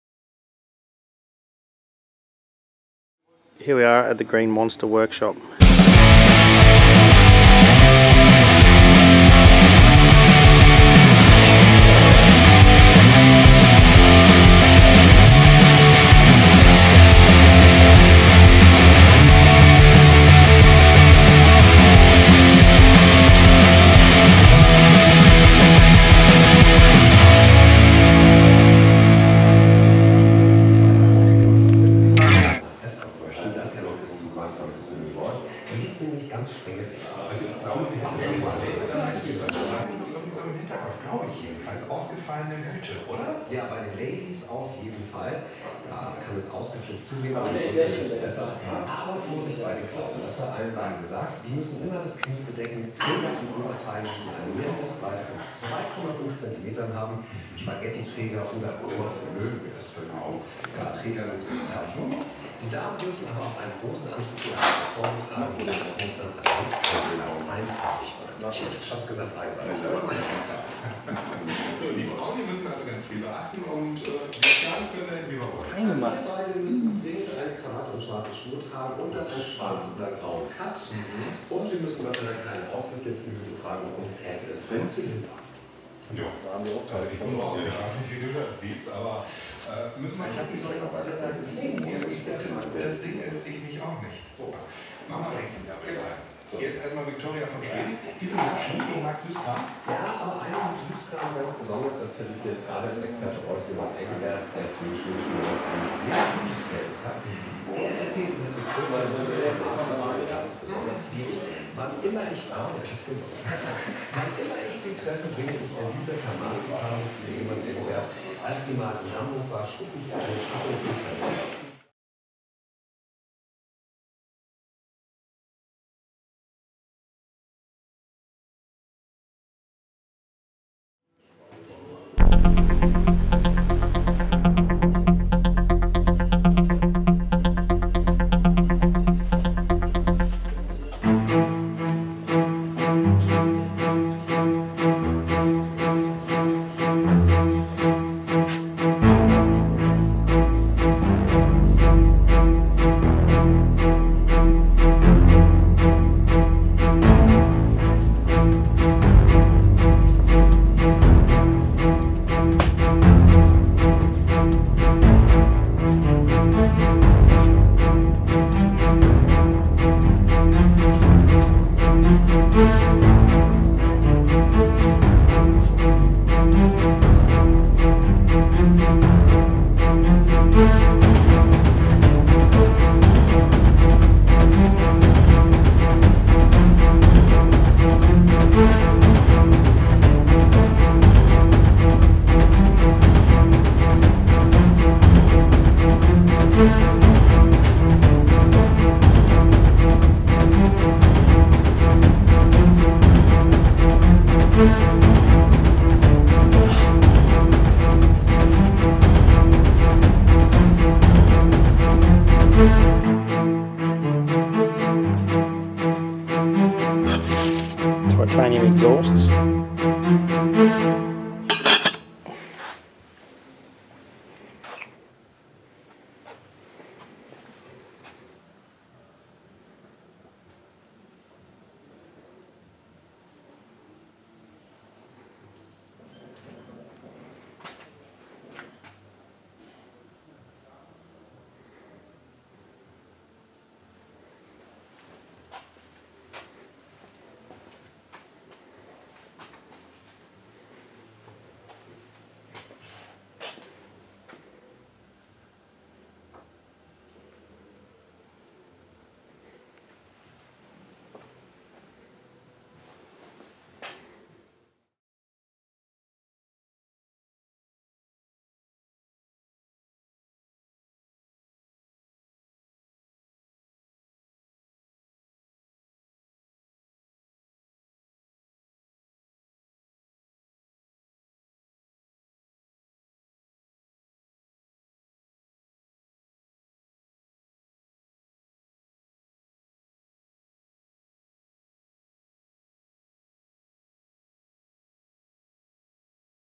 Green Monster Tractor Pulling Team
Green Monster Workshop Tour 20 June 2019
I was so lucky to have had the chance to go through one of the biggest pulling teams in Europe. Green Monster and Green Fighter clearly crowd favorites​.